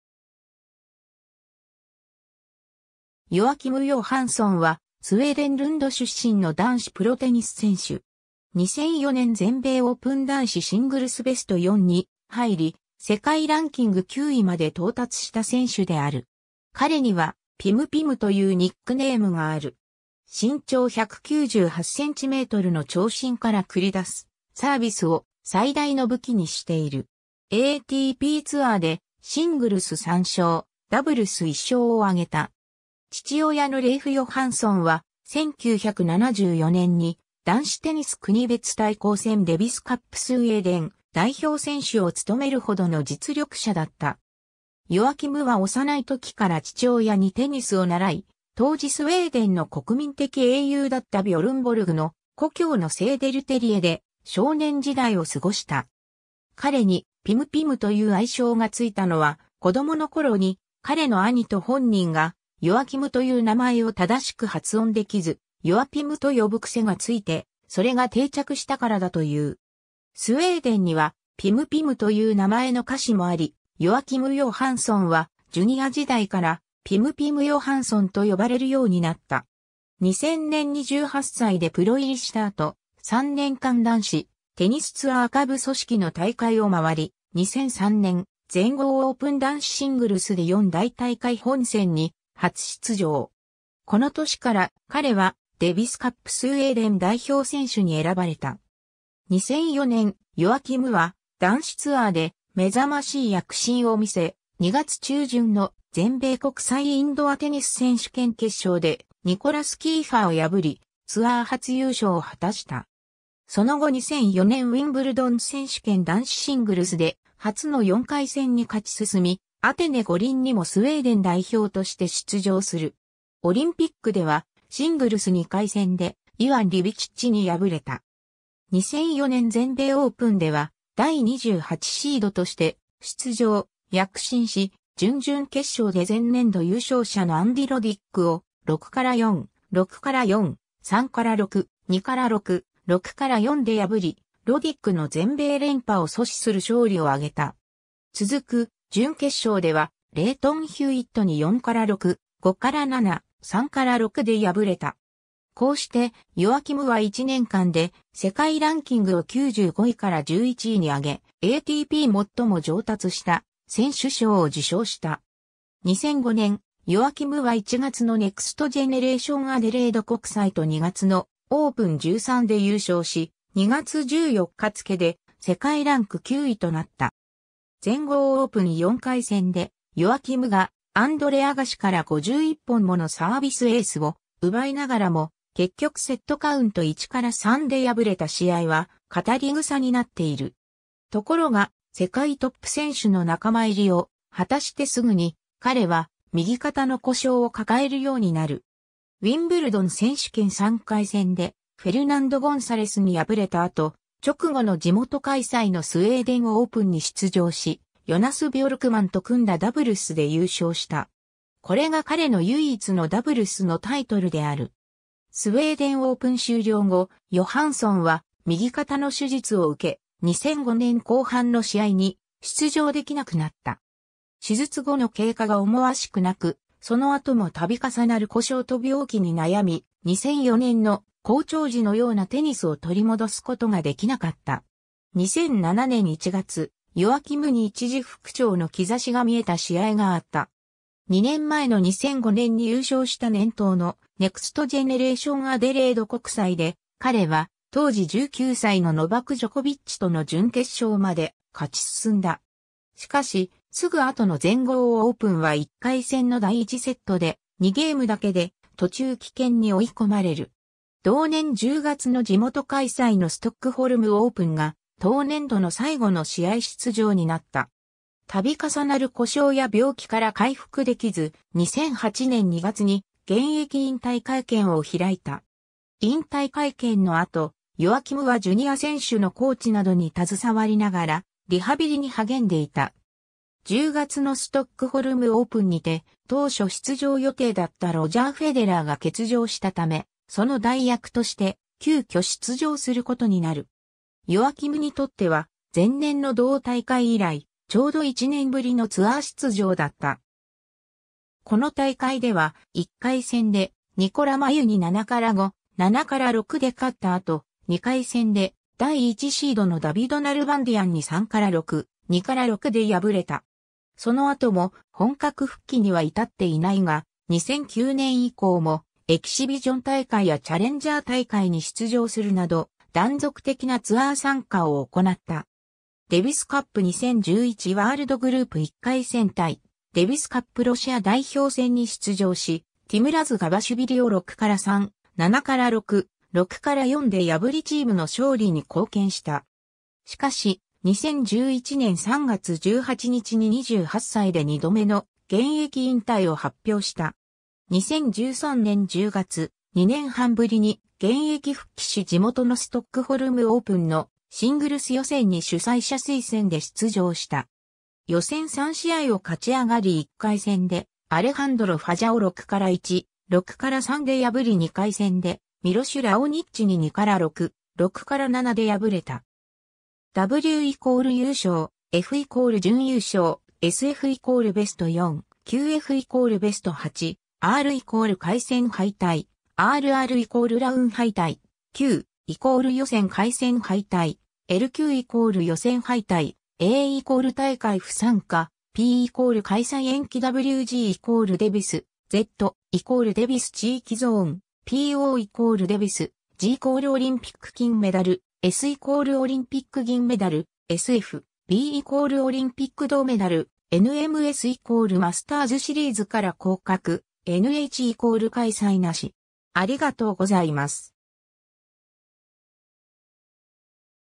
ヨアキム・ヨハンソン
ヨアキム・ヨハンソン（Joachim Johansson, 1982年7月1日 - ）は、スウェーデン・ルンド出身の男子プロテニス選手。
2004年全米オープン男子シングルスベスト4に入り、世界ランキング9位まで到達した選手である。
彼には「ピム・ピム」（Pim-Pim）というニックネームがある。
身長198cmの長身から繰り出すサービスを最大の武器にしている。
ATPツアーでシングルス3勝、ダブルス1勝を挙げた。
 父親のレイフ・ヨハンソンは、1974年に男子テニス国別対抗戦デビスカップスウェーデン代表選手を務めるほどの実力者だった。
 ヨアキムは幼い時から父親にテニスを習い、当時スウェーデンの国民的英雄だったビョルン・ボルグの故郷のセーデルテリエで少年時代を過ごした。
彼に「ピム・ピム」という愛称がついたのは、子供の頃に彼の兄と本人が「ヨアキム(Joachim)」という名前を正しく発音できず「ヨアピム(Joa-pim)」と呼ぶ癖がついて、それが定着したからだという。
スウェーデンには「ピム・ピム」という名前の菓子もあり、ヨアキム・ヨハンソンはジュニア時代から「ピム・ピム・ヨハンソン」と呼ばれるようになった。
2000年に18歳でプロ入りした後、3年間男子テニスツアー下部組織の大会を回り、2003年全豪オープン男子シングルスで4大大会本戦に初出場。
この年から、彼はデビスカップスウェーデン代表選手に選ばれた。
 2004年、ヨアキムは男子ツアーで目覚ましい躍進を見せ、2月中旬の全米国際インドアテニス選手権決勝でニコラス・キーファーを破り、ツアー初優勝を果たした。
その後2004年ウィンブルドン選手権男子シングルスで初の4回戦に勝ち進み、アテネ五輪にもスウェーデン代表として出場する。
オリンピックでは、シングルス2回戦でイワン・リュビチッチに敗れた。
2004年全米オープンでは第28シードとして出場・躍進し、準々決勝で前年度優勝者のアンディ・ロディックを6-4, 6-4, 3-6, 2-6, 6-4で破り、ロディックの全米連覇を阻止する勝利を挙げた。
続く準決勝ではレイトン・ヒューイットに4-6, 5-7, 3-6で敗れた。
こうして、ヨアキムは1年間で世界ランキングを95位から11位に上げ、ATP最も上達した選手賞を受賞した。
 2005年、ヨアキムは1月のネクストジェネレーション・アデレード国際と2月のオープン13で優勝し、2月14日付で世界ランク9位となった。
全豪オープン4回戦で、ヨアキムがアンドレ・アガシから51本ものサービスエースを奪いながらも、結局セットカウント1-3で敗れた試合は語り草になっている。
 ところが、世界トップ選手の仲間入りを果たしてすぐに、彼は右肩の故障を抱えるようになる。
ウィンブルドン選手権3回戦でフェルナンド・ゴンサレスに敗れた後、直後の地元開催のスウェーデン・オープンに出場し、ヨナス・ビョルクマンと組んだダブルスで優勝した。
これが彼の唯一のダブルスのタイトルである。
スウェーデン・オープン終了後、ヨハンソンは右肩の手術を受け、2005年後半の試合に出場できなくなった。
手術後の経過が思わしくなく、その後も度重なる故障と病気に悩み、2004年の好調時のようなテニスを取り戻すことができなかった。
 2007年1月、ヨアキムに一時復調の兆しが見えた試合があった。
2年前の2005年に優勝した年頭のネクストジェネレーション・アデレード国際で、彼は当時19歳のノバク・ジョコビッチとの準決勝まで勝ち進んだ。
しかし、すぐ後の全豪オープンは1回戦の第1セットで、2ゲームだけで途中棄権に追い込まれる。
同年10月の地元開催のストックホルム・オープンが、当年度の最後の試合出場になった。
度重なる故障や病気から回復できず、2008年2月に現役引退会見を開いた。
 引退会見の後、ヨアキムはジュニア選手のコーチなどに携わりながらリハビリに励んでいた。
10月のストックホルム・オープンにて、当初出場予定だったロジャー・フェデラーが欠場したため、その代役として急遽出場することになる。
ヨアキムにとっては、前年の同大会以来ちょうど1年ぶりのツアー出場だった。
この大会では、1回戦でニコラ・マユに7-5, 7-6で勝った後、2回戦で第1シードのダビド・ナルバンディアンに3-6, 2-6で...